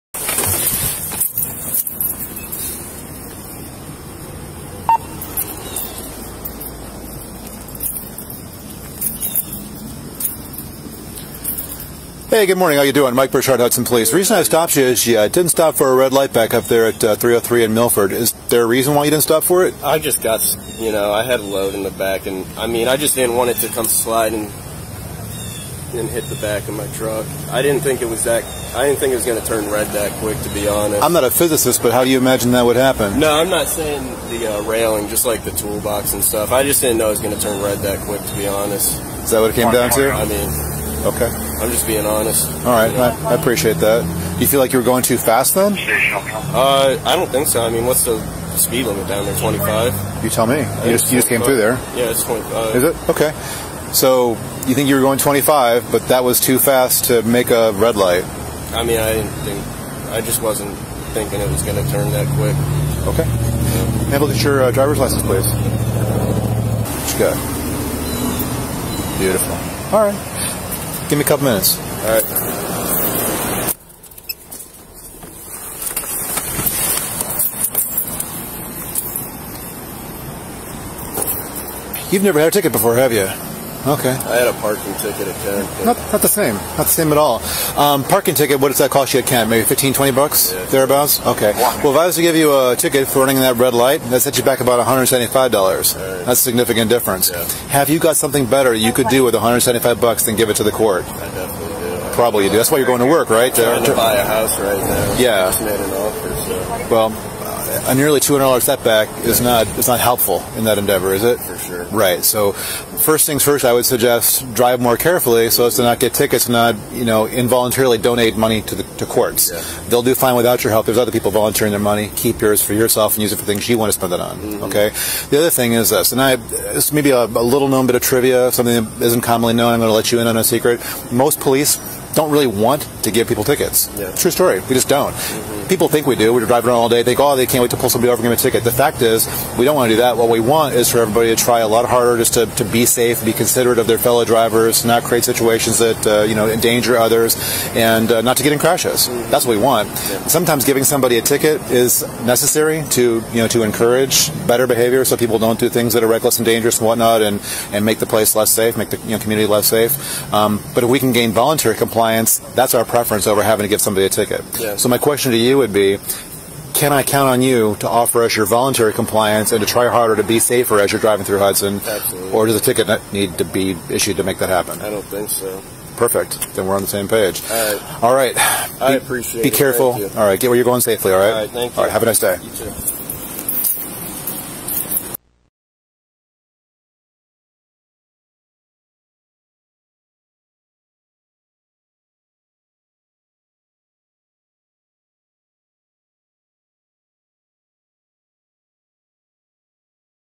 BODYCAM Officer Shows Mercy To Man Pulled Over
When this man is pulled over, an officer lets him off with a lesson instead of a ticket.